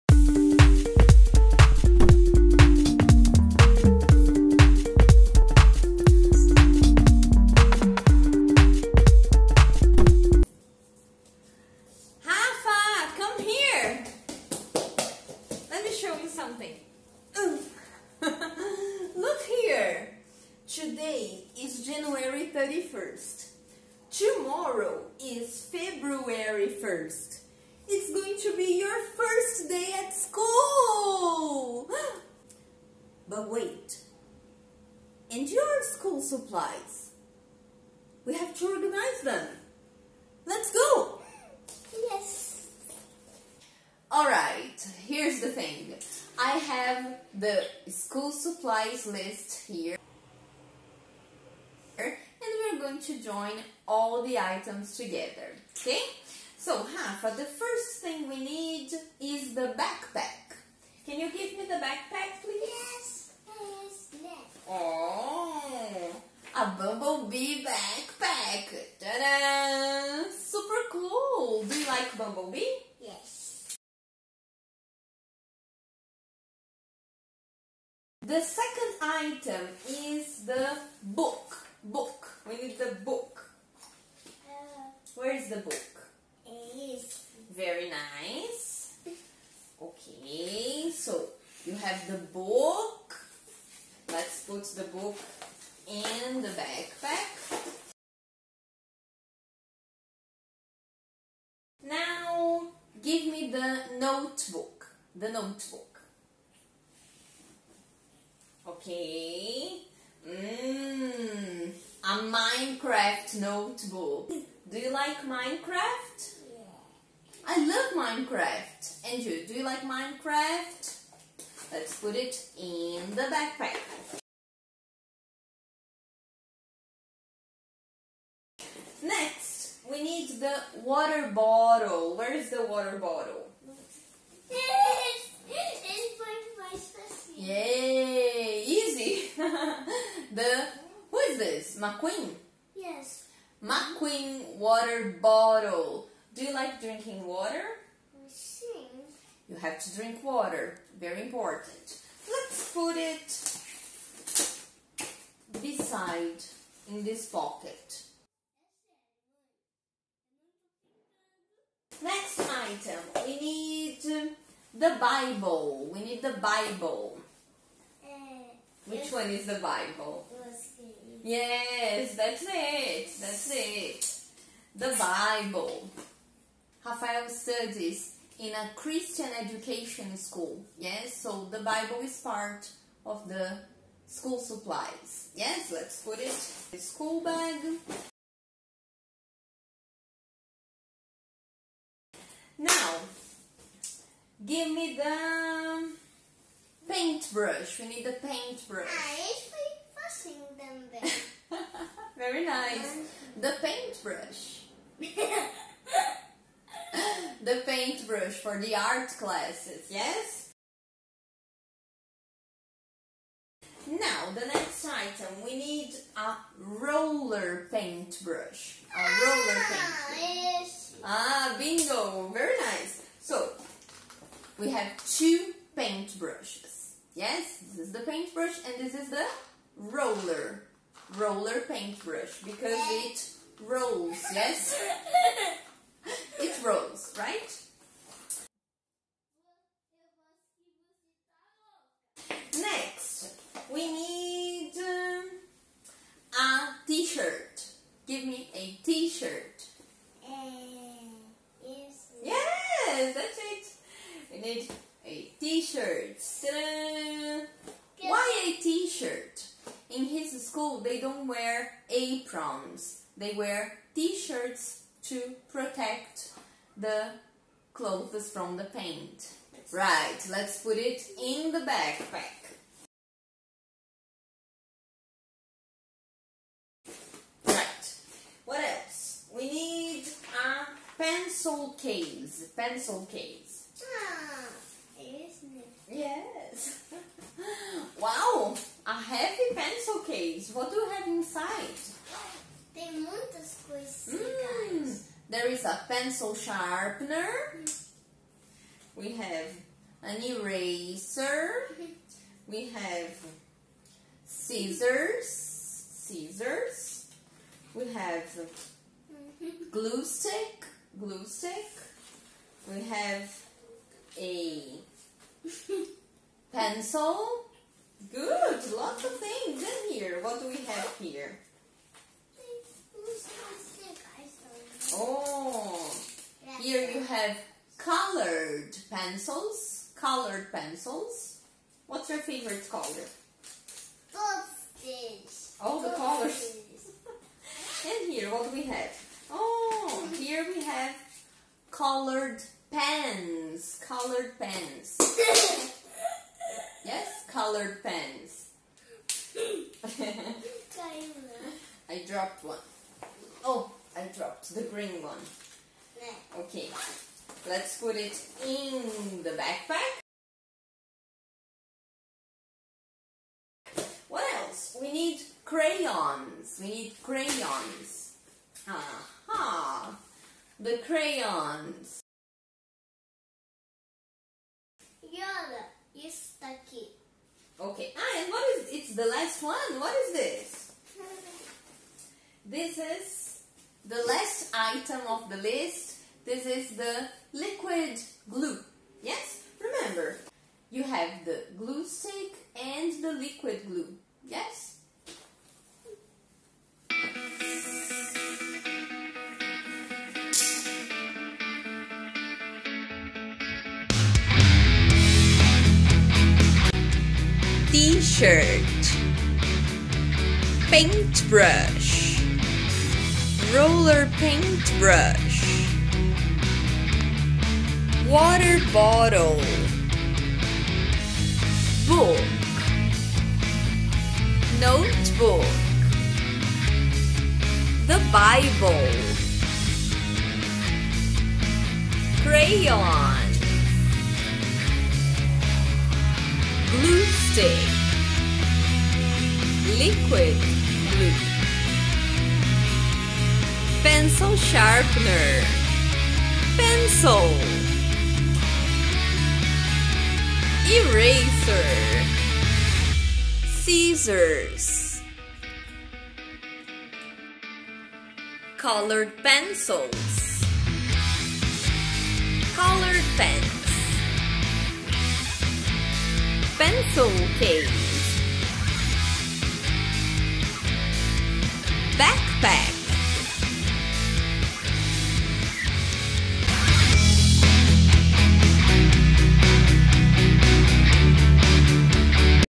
School Supplies - Bilingual Class
Nesta aula, organizamos nosso material escolar!

Compartilhe essa aula! Espalhe conhecimento 😊

Vivência, sim! Tradução, não! Venha aprender inglês conosco!